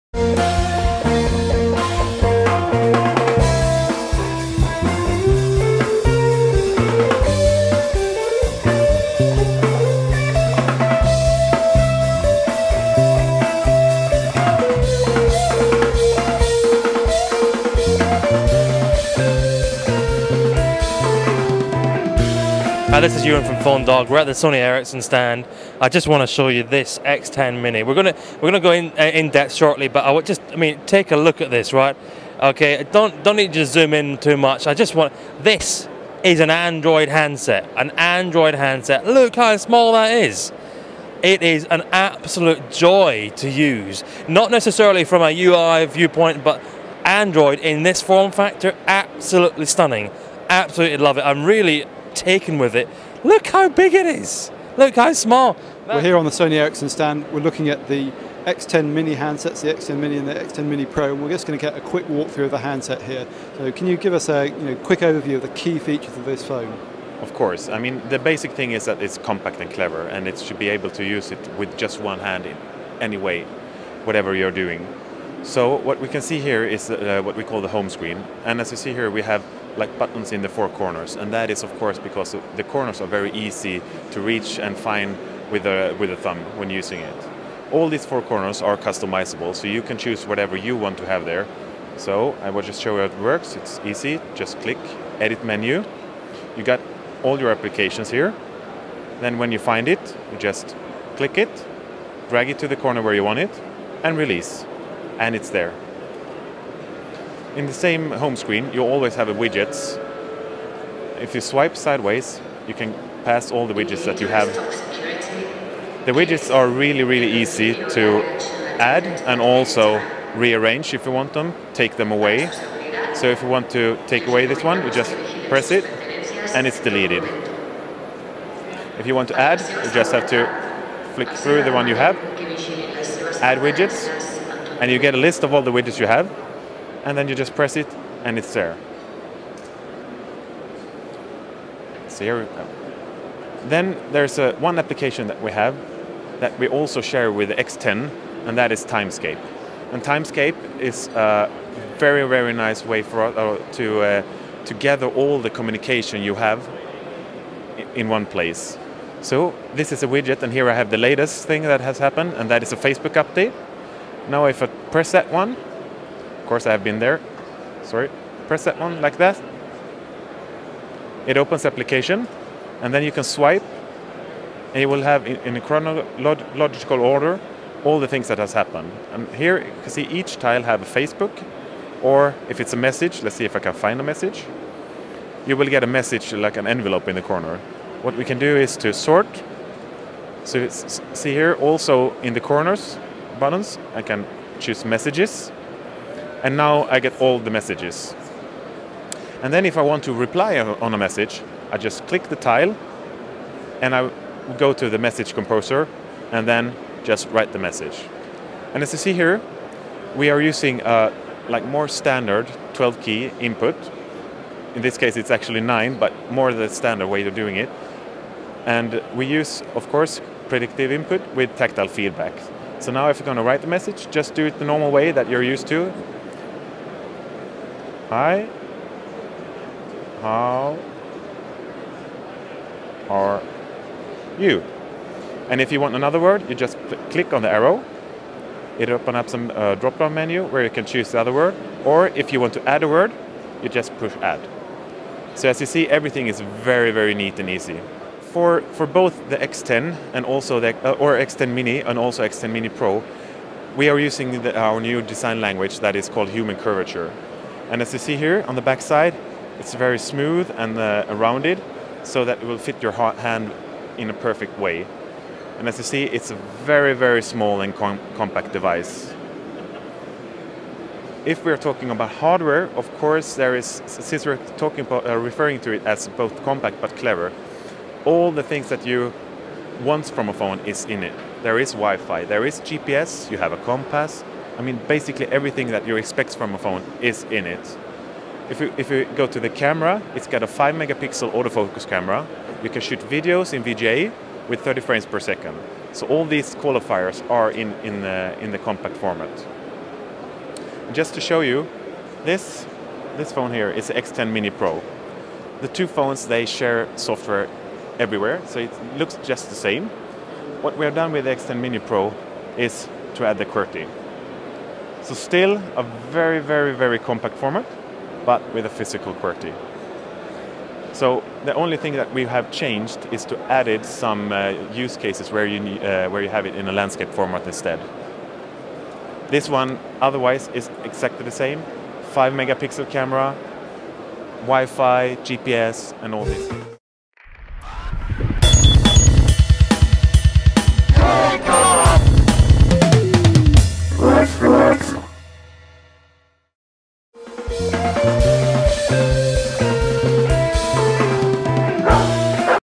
Sony Ericsson X10 mini and X10 mini Pro - Hands-On
Hands-on with SE's new Android phones: Xperia X10 mini and X10 mini Pro.  Live from Mobile World Congress 2010 in Barcelona.

More Videos for all the latest cell phone news, reviews, and more.